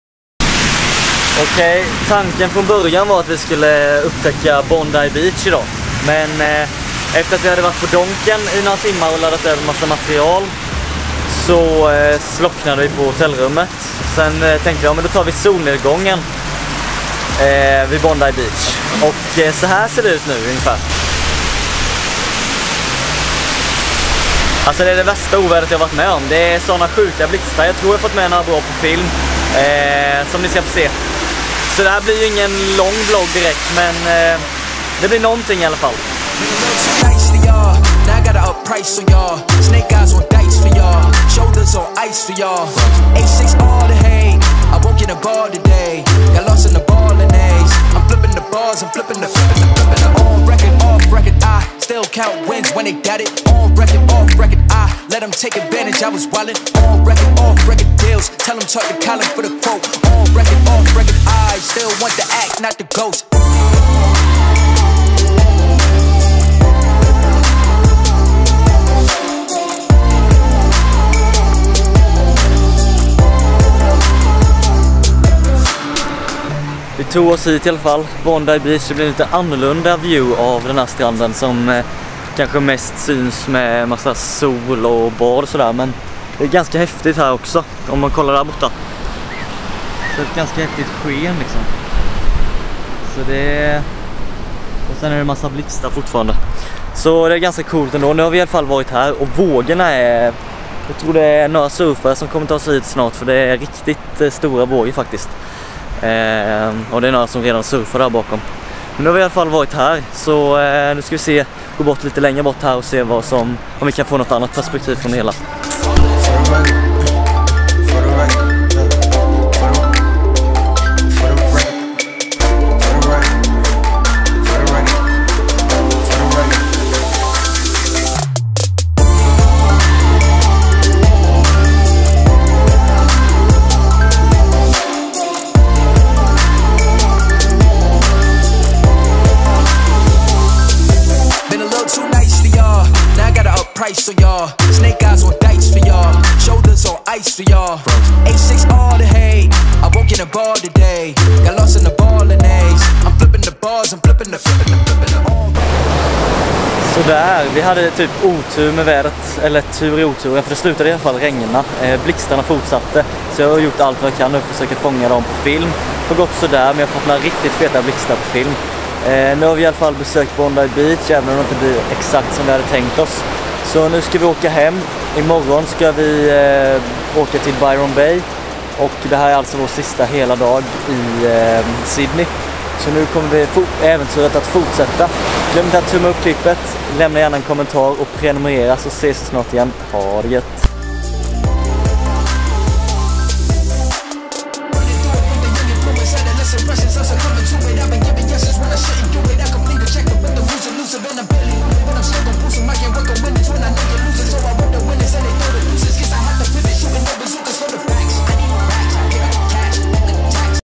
Australien - Mörka sidan av Bondi Beach - Vlogg 5
Vi bestämde oss för att besöka Bondi Beach och det blev inte riktigt som vi tänkt oss. Blixtar, oska och skyfall började plötsligt påvägen till stranden... En upplevelse i sig men inte vad vi hade räknat med.

Edit: Kim Petersson @Kpeterssonphoto
Behind Camera: Albin Jonsson

Camera: GH5